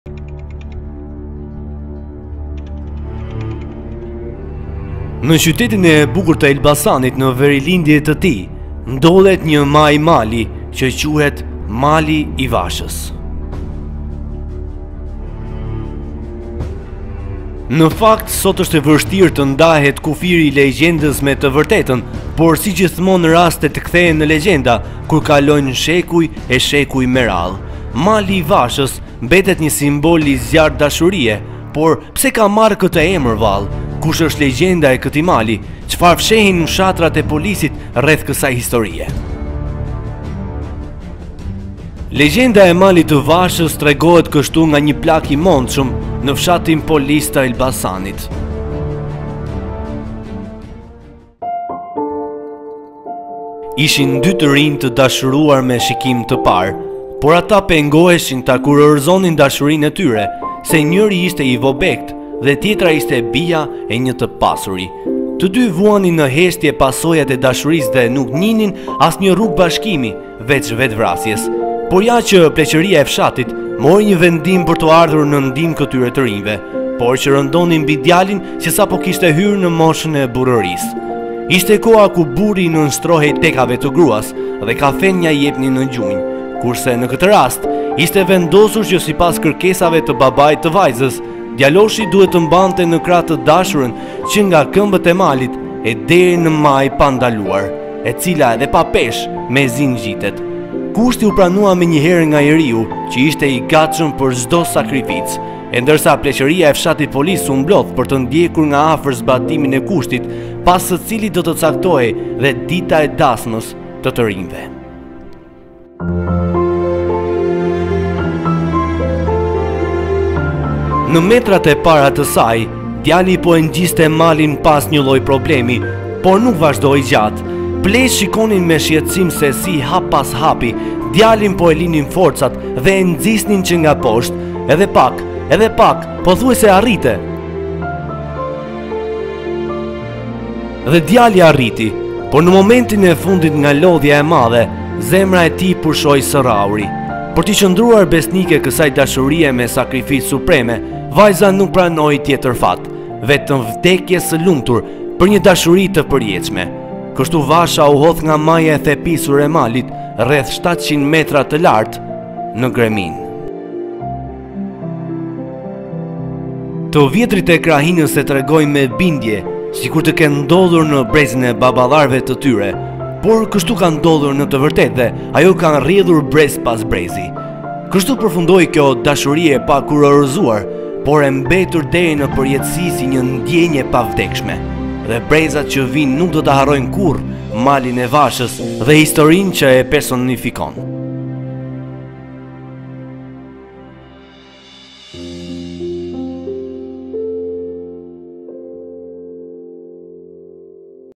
Legjenda e "Malit të Vashës"
Në qytetin e bukur të Elbasanit në verilindje të tij, ndot një majë mali që quhet “Mali i Vashës”.
Në fakt sot është e vështirë të ndahet kufiri i legjendës më të vërtetën por si gjithmonë rastet kthehen në legjenda kur kalojnë shekuj e shekuj me radhë.
“Mali i Vashës” mbetet një simbol i një dashurie të zjarrtë. Pse ka marrë këtë emër vallë? Kush është legjenda e këtij mali? Çfarë fshehin fshatarët e Polisit rreth kësaj historie? Shikoni videon e realizuar nga Daniel Mucaku.